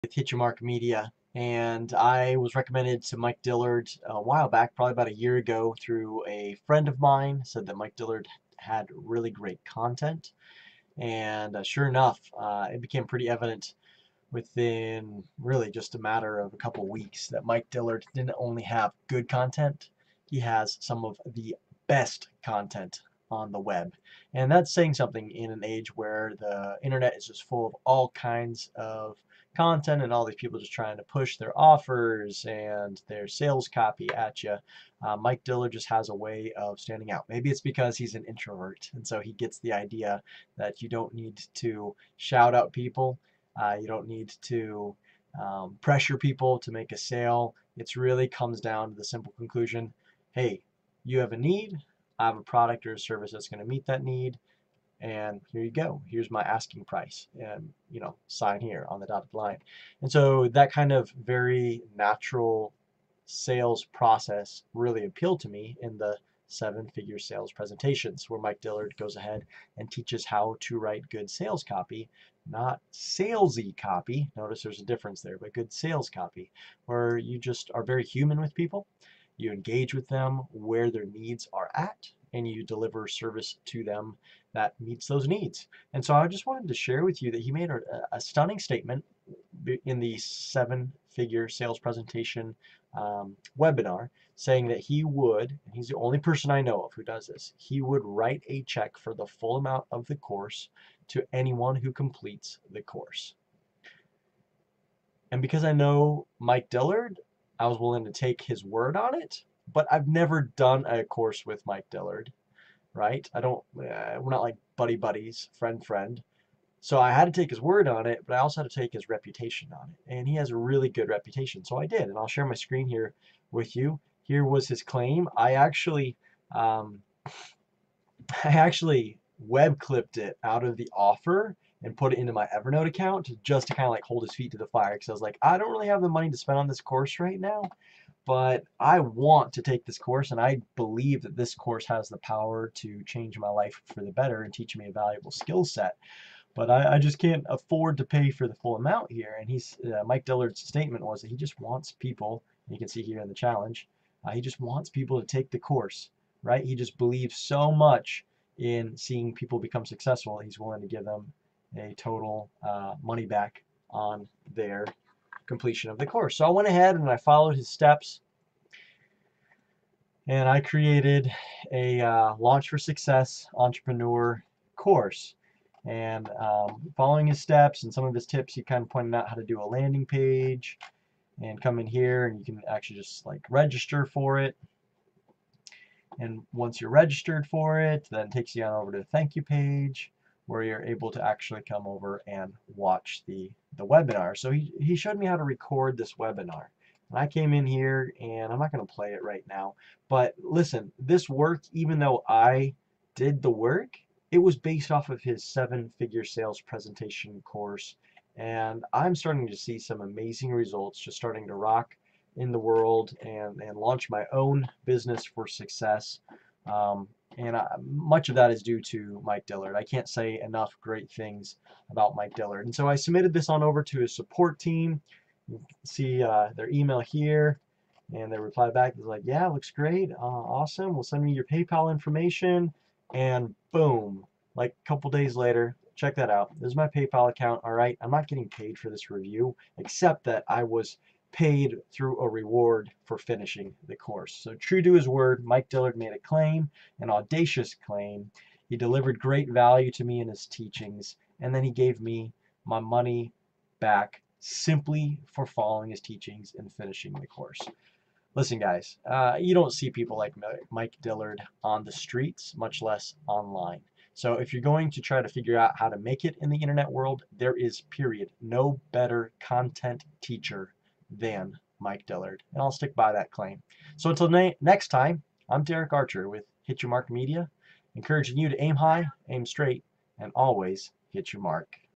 Mike Dillard's 7-Figure Sales course worth it??
Derek Archer's review of Mike Dillard's 7-Figure Sales Presentations Master Class.

Learn how to build your first successful online business from Mike, step-by-step, in just 52 days

Mike